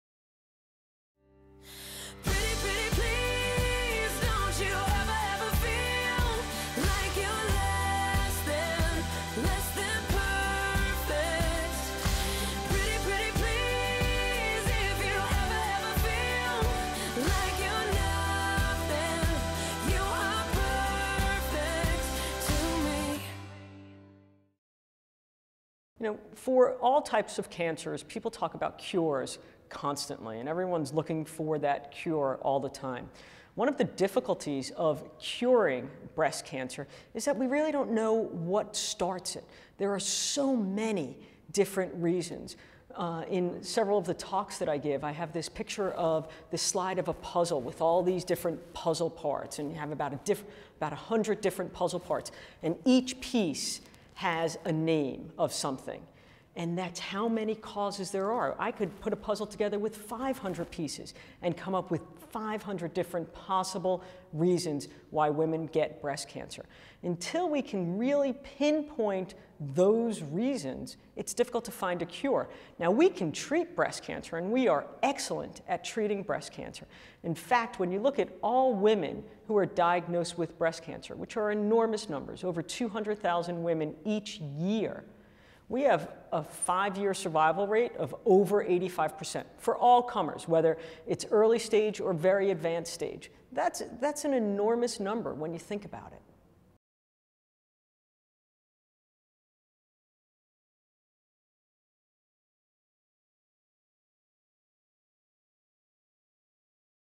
Breast Cancer Discoveries Day 1: Why Finding a Cure is Difficult
People talk about a cures for cancer constantly and everyone's looking for that cure all the time.  One of the difficulties of curing breast cancer is that we really don't know what starts it.

In this video, Dr. Susan K. Boolbol, Chief Breast Cancer Surgeon at Beth Israel Medical Center in New York City, explains the many reason why finding a cure is so difficult.